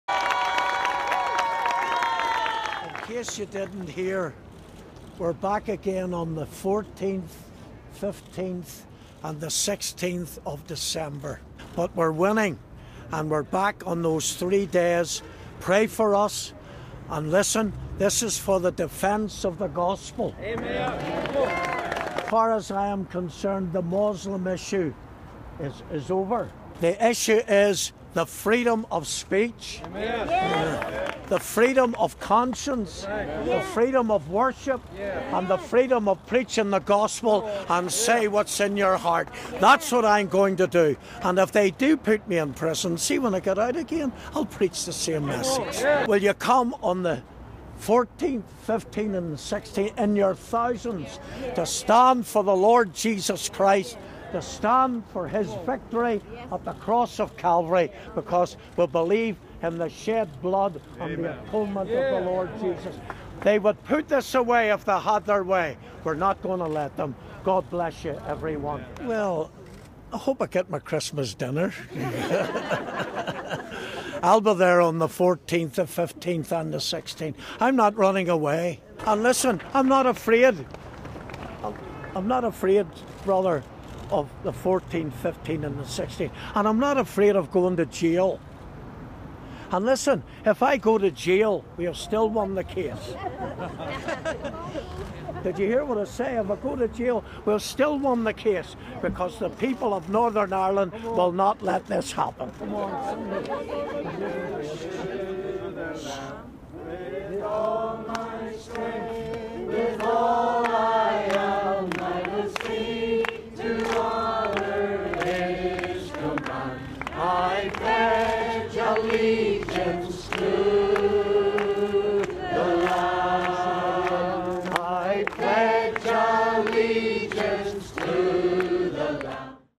Pastor McConnell speaks outside Laganside Court, Belfast
By Adrian Rutherford

Lawyers have started a court bid to summon BBC presenter Stephen Nolan as a witness in the trial of a pastor facing charges over an anti-Islamic sermon, branding the presenter's initial refusal to attend as "the biggest no-show in the country".

Pastor James McConnell's defence team want to call Nolan to give evidence.

But today it emerged Nolan has responded - through the BBC's legal team - to say he will only attend court if compelled to do so.

It led solicitor Joe Rice to joke that Nolan's unwillingness to appear was "the biggest no-show in the country".

A judge said he would consider the request for Nolan to be summoned and respond at a later date.

Pastor McConnell is charged with improper use of a public electronic communications network, and causing a grossly offensive message to be sent by means of a public electronic communications network.

The charges related to remarks made in an internet-broadcast sermon he delivered at his Whitewell Metropolitan Tabernacle in north Belfast on May 18 last year.

He said "a new evil had arisen" and "there are cells of Muslims right throughout Britain".

"Islam is heathen, Islam is satanic, Islam is a doctrine spawned in hell," he said.

Pastor McConnell was later questioned by police about his remarks.

He issued a public apology for any offence caused.

However, earlier this year it emerged he would be prosecuted over the comments.

Mr Nolan carried out a number of television and radio interviews with Pastor McConnell in the wake of the sermon last May.

The case has been listed for December 14 and is expected to last three days.